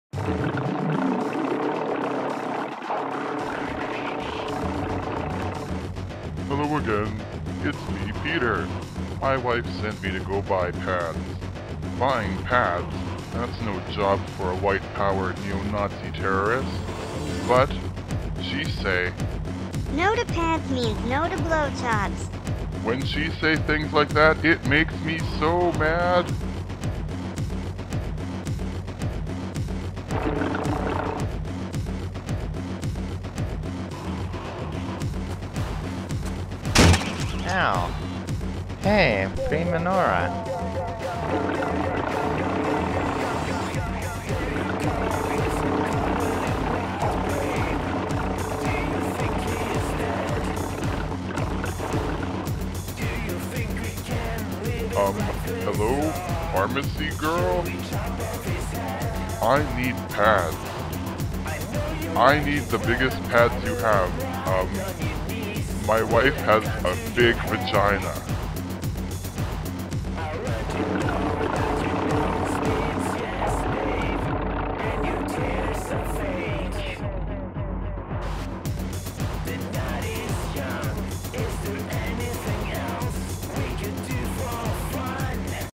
Skinhead's Video Blog - Pads!
Skinhead is sent on an errand by his wife. Wacky hijinx ensue